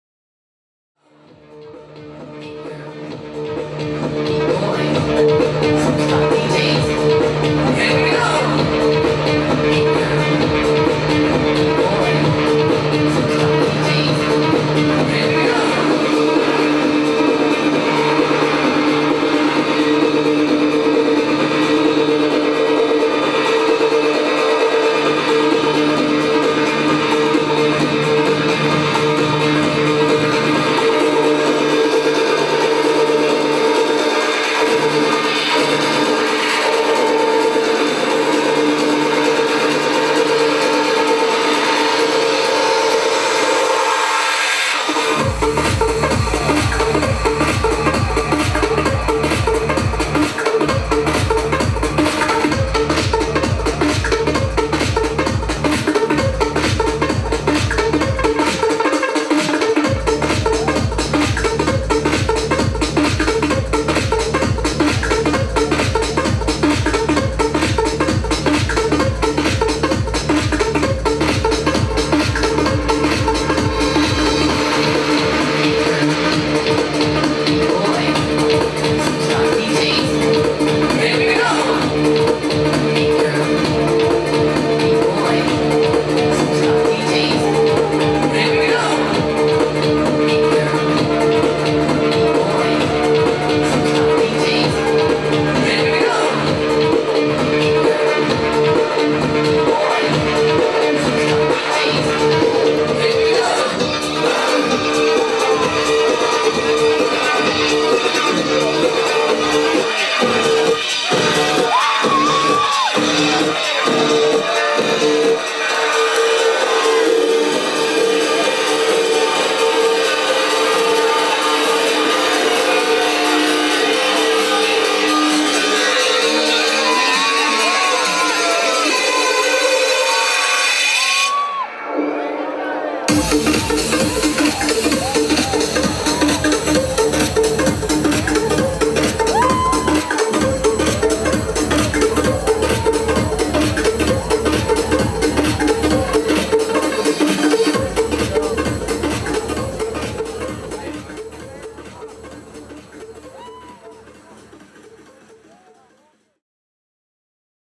Tommy Trash @ BW Campgrounds, Dec 27th 2009
Tommy Trash @ BW Camping Festival, December 27th 2009, performing live at the Jim Beam Soundshell.